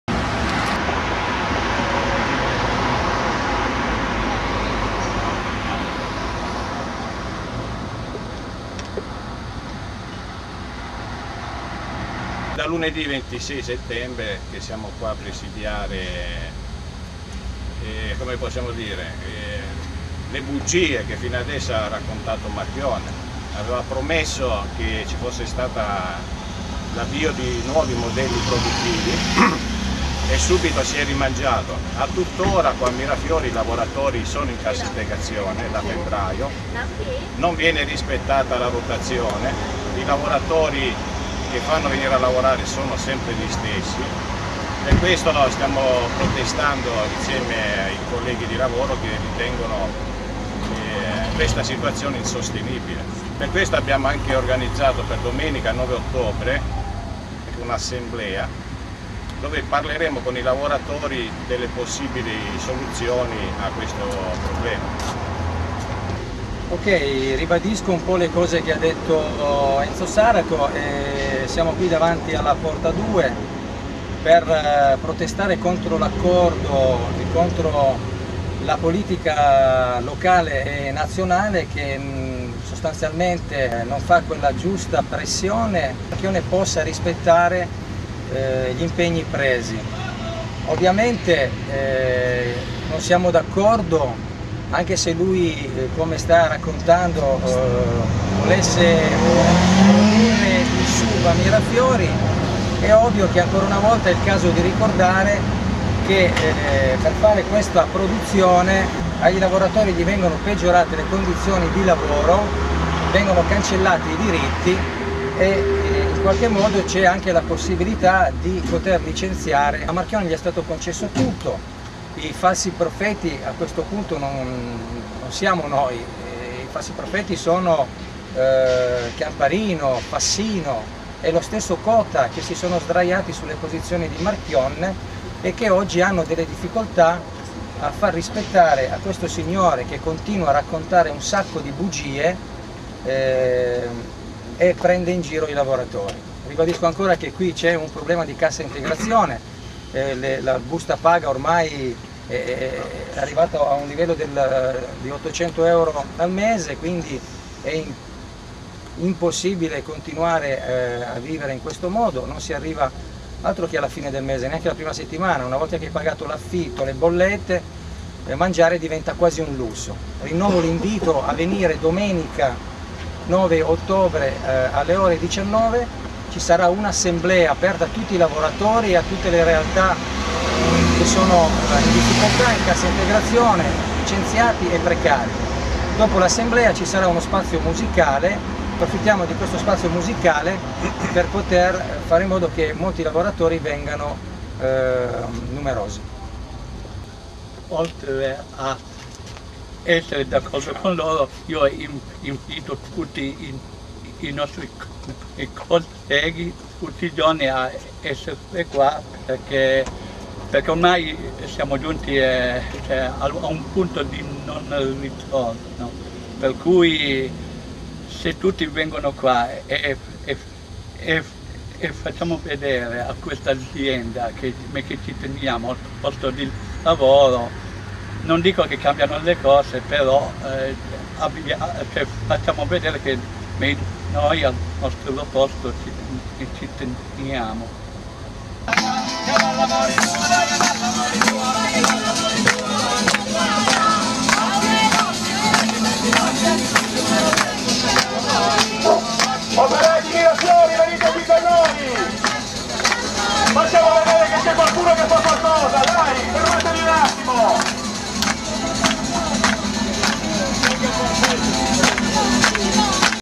Intervista ai cassaintegrati del presidio alla Fiat Mirafiori
Intervista, effettuata domenica pomeriggio 2 ottobre, di cassaintegrati Fiat Mirafiori che spiegano le loro ragioni, i motivi del presidio e lanciano un'iniziativa per domenica prossima 9 ottobre alle ore 19 davanti alla porta 2 , in corso Tazzoli.